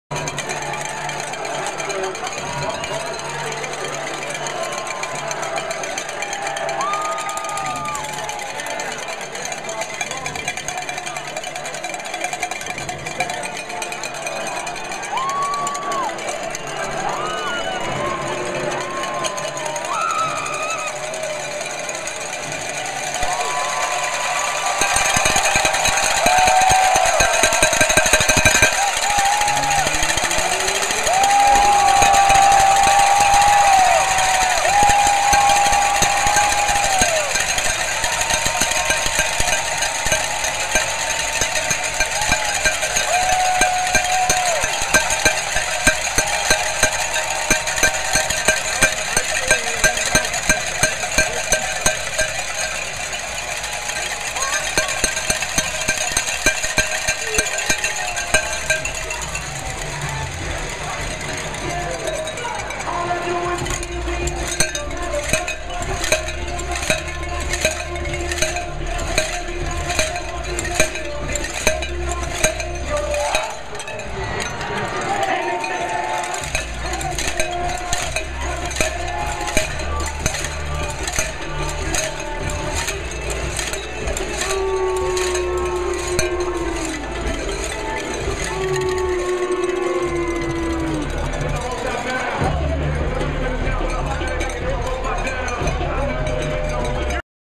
UK vs MSU -- Pregame Team Run Out
University of Kentucky vs Mississippi State University in Starkville, MS. Team entrances before kick off from the student section.
30 October 2010